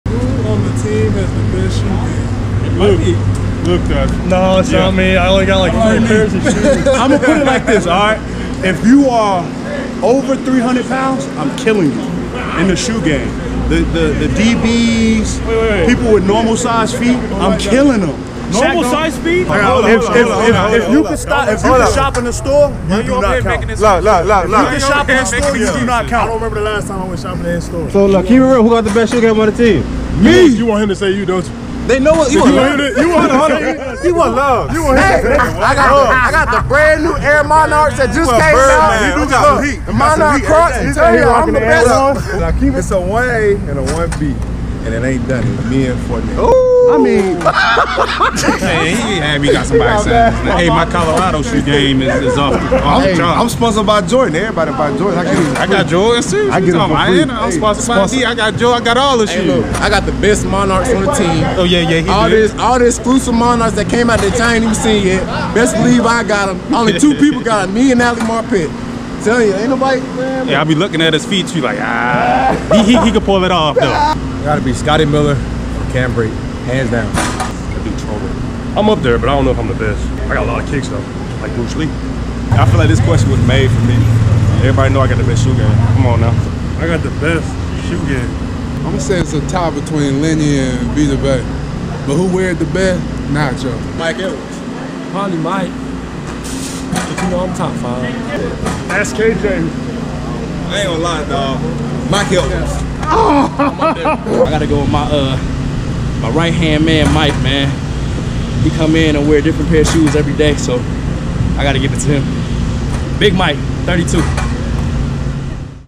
Who On The Team Has The Best Shoe Game? | Practice Cam
Watch as players answer the question: who on the team has the best shoe game?